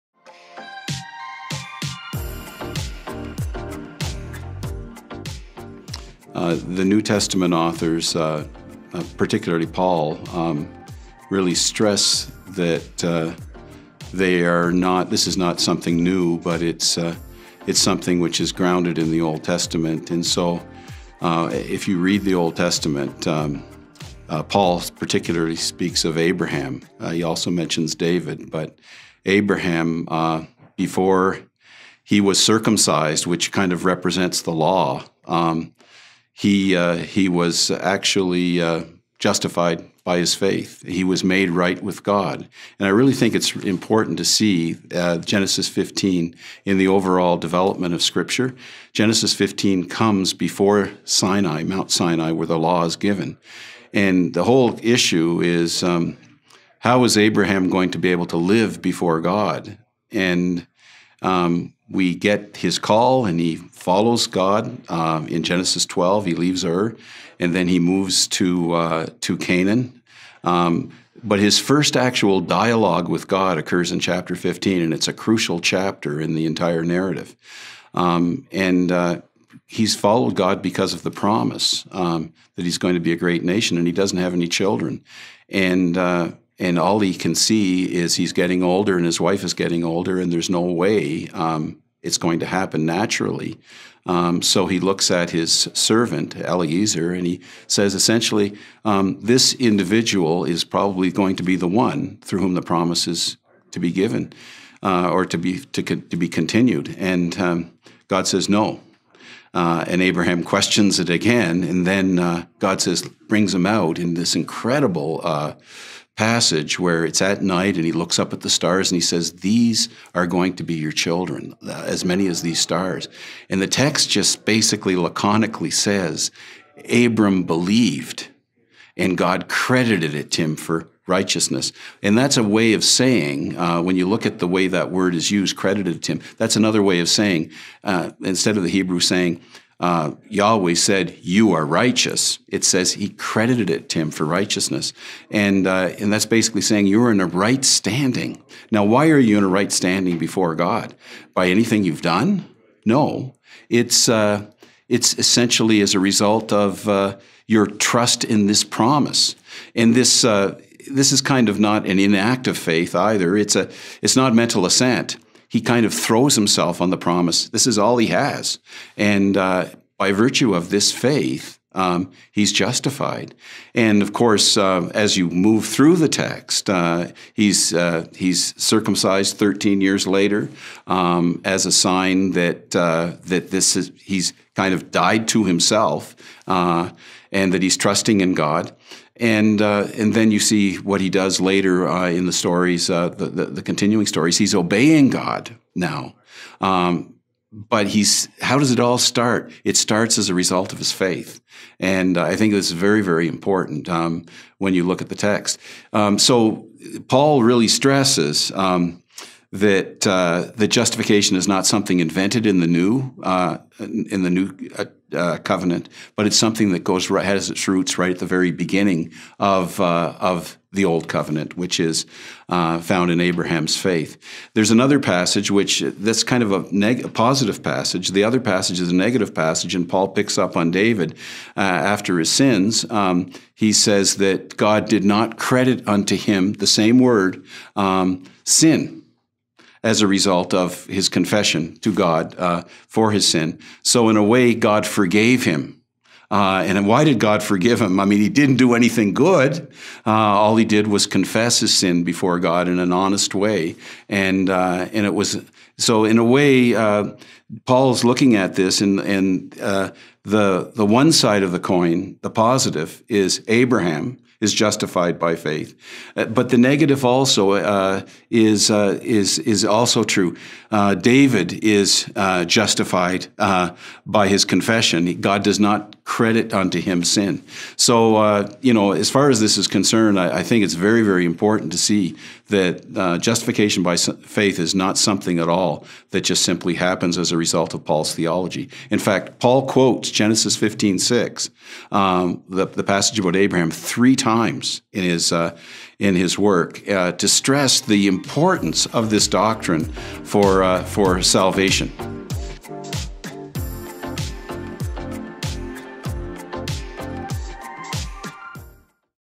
Is the doctrine of justification in the Old Testament?
Where is justification visible in the Old Testament? How should we think about the connection between faith and works in the life of Abraham? Should we consider Abraham a type of new Adam and if so, how does he point us forward to Christ as the last Adam? Is justification lost on the prophets? In this new Credo video, Stephen Dempster explains how the doctrine of justification by faith alone in Christ alone is present in the Old Testament.